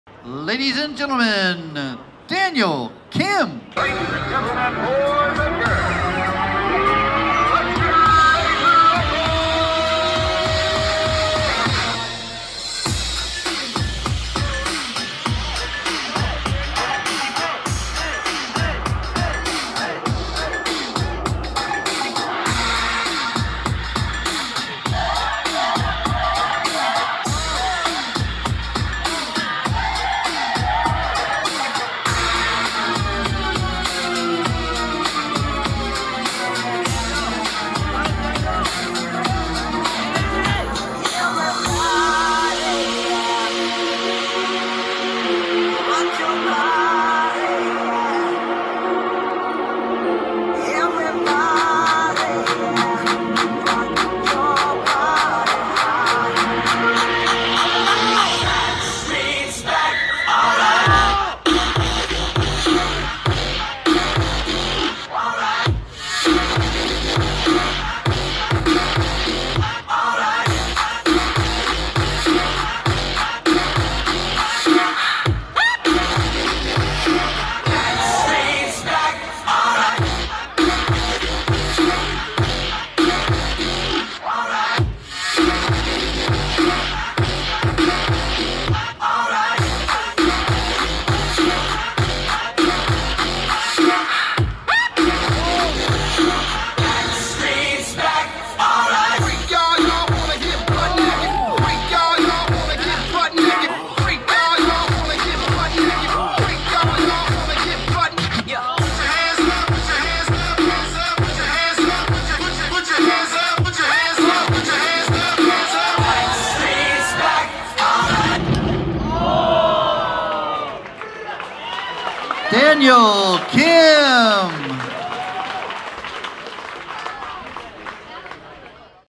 Daniel Kim - 1A Final - 12th Place - BAC 2017 - Presented by Yoyo Contest Central
The 2017 Bay Area Classic Yoyo Contest was held on June 3rd at the Children's Discovery Museum in San Jose, CA. Presented by the National YoYo League, this event was made possible by the following sponsors:

JD Events